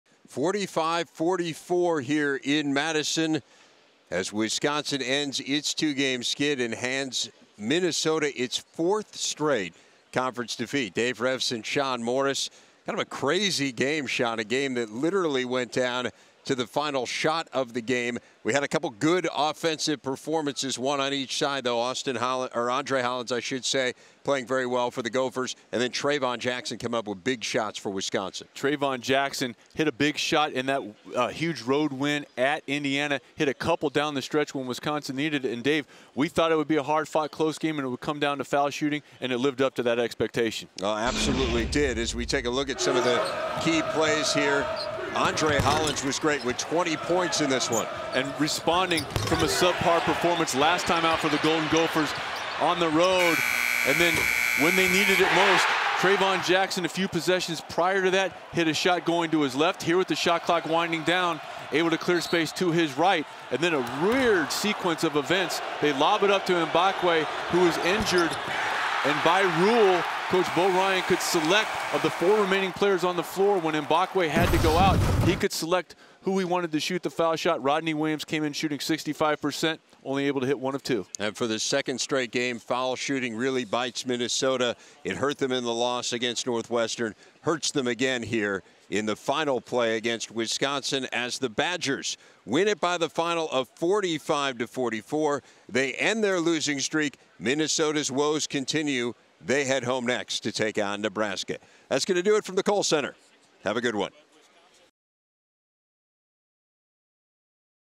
BTN Recap: Gopher Basketball Falls to Wisconsin 45-44 in Madison
Recap from the Big Ten Network of Minnesota men's basketball's 45-44 loss to Wisconsin in Madison on Jan. 26, 2013